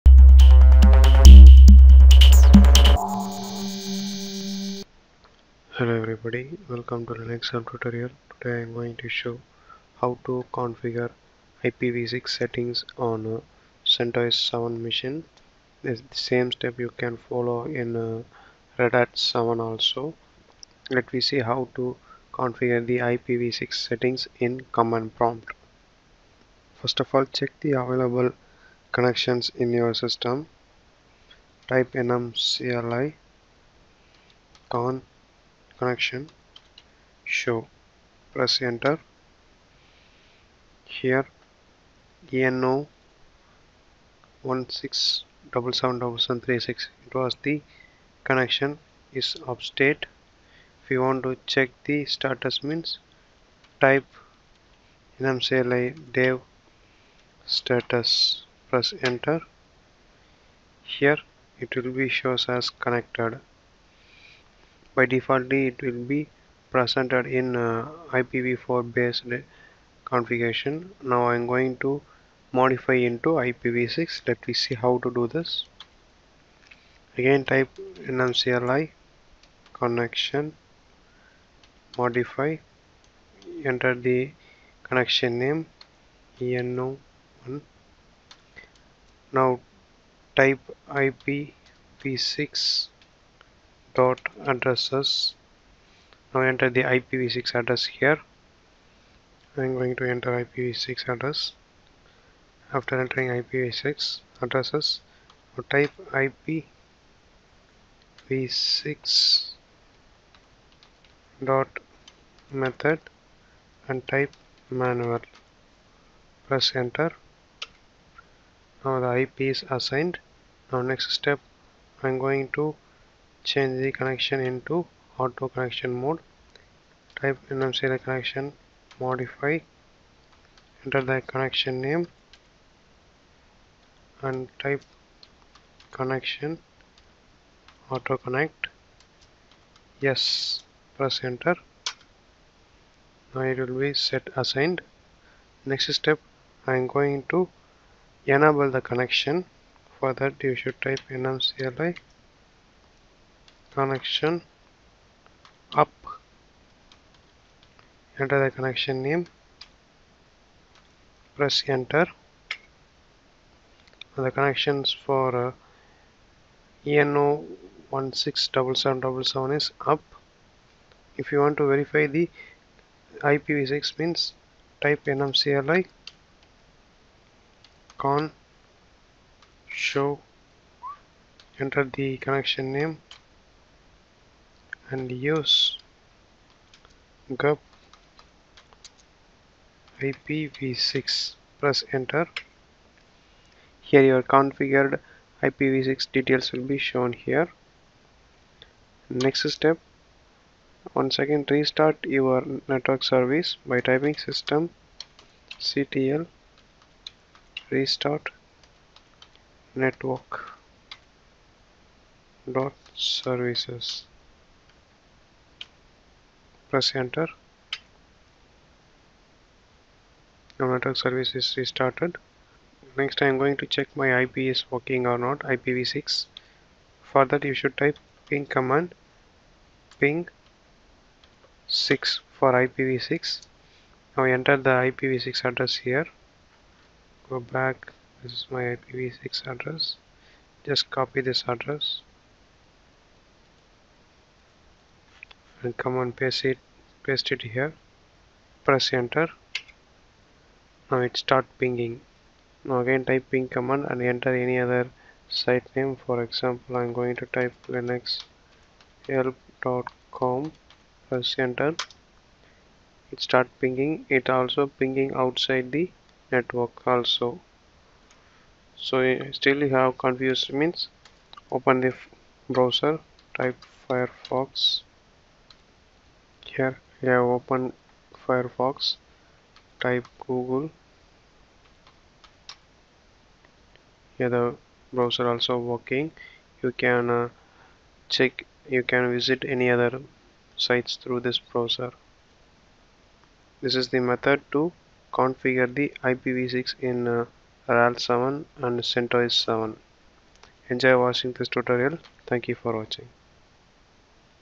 How to configure IPV6 address in Centos Via Command Prompt
This video explains about the methods to configure IPv6 on CentOS. IPv6 is an updated and recent version of internet protocol.